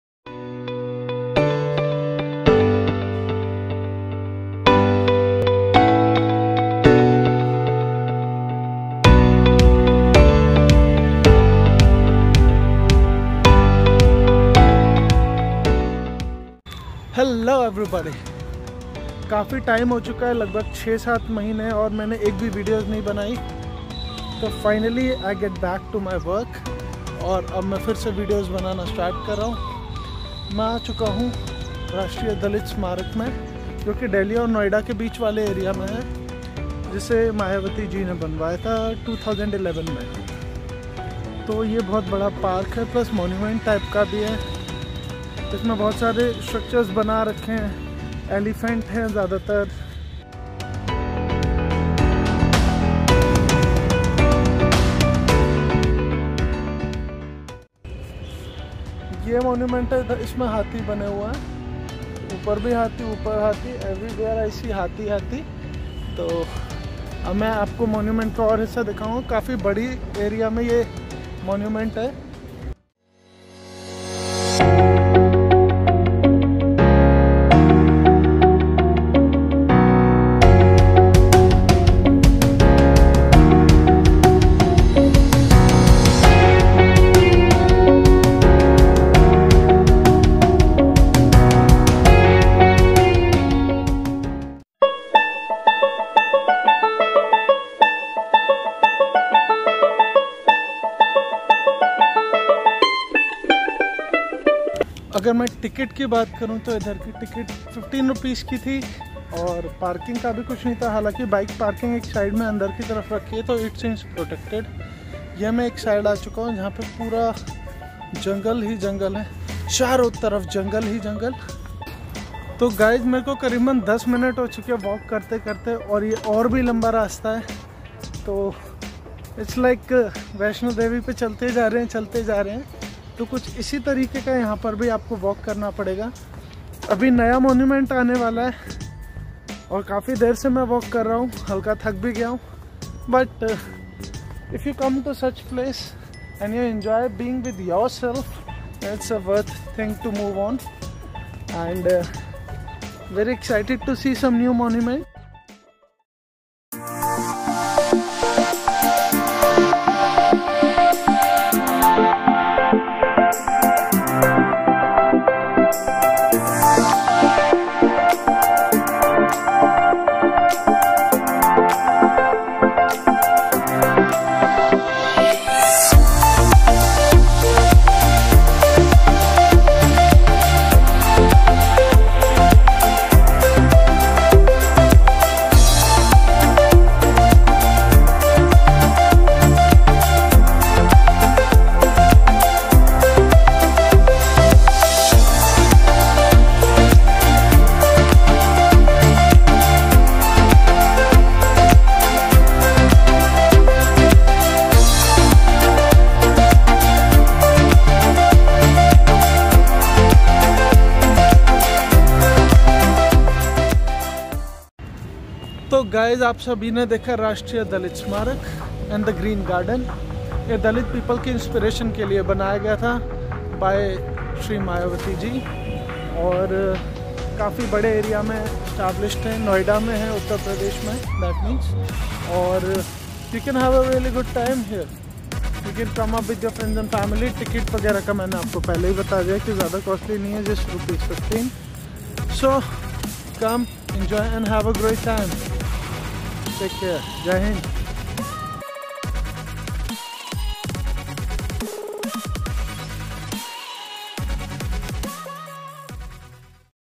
Travel with me visit at Rashtriya Dalit Smarak
Rashtriya Dalit Prerna Sthal and Green Garden. The Rashtriya Dalit Prerna Sthal and Green Garden (National Dalit Inspiration Place and Green Garden) is a memorial in Noida, Uttar Pradesh, India. It was commissioned by Chief Minister of Uttar Pradesh Mayawati(2011) and inaugurated on 14 October 2011